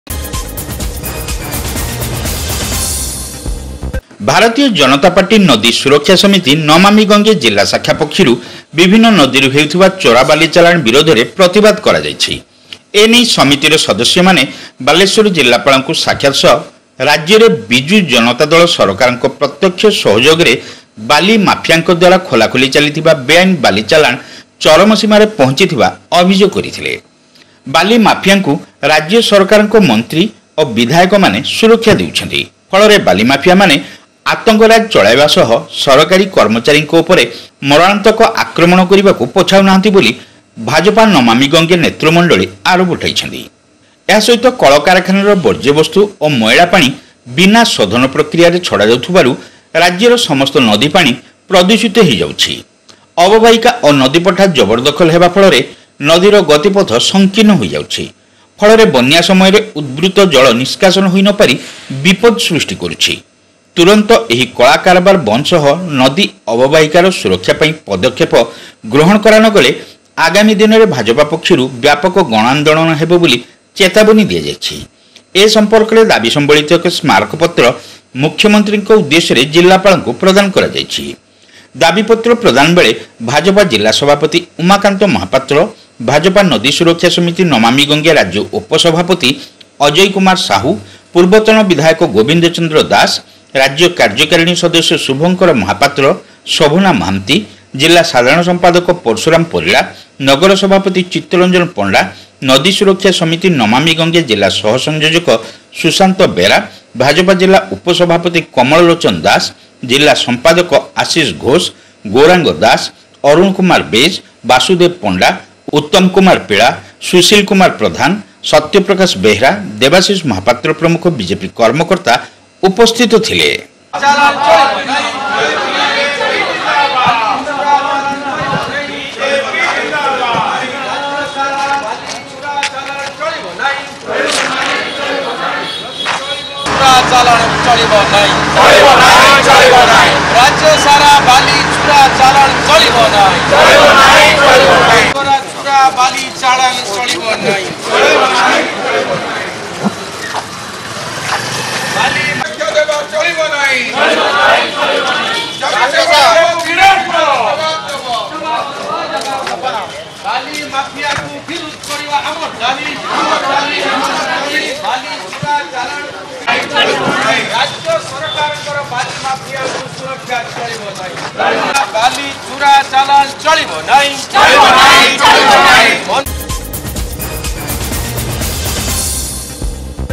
ମାଧବ ନ୍ୟୁଜ - ବାଲେଶ୍ଵରରେ ବିଜେପି ନମାମି ଗଙ୍ଗେ ସମିତିର ବେଆଇନ ବାଲି ଚାଲାଣ ବିରୋଧରେ ପ୍ରତିବାଦ
Madhab News - BJP Namami Gange River Protection Committee Protest at Balasore Against Illegal Sand Mining - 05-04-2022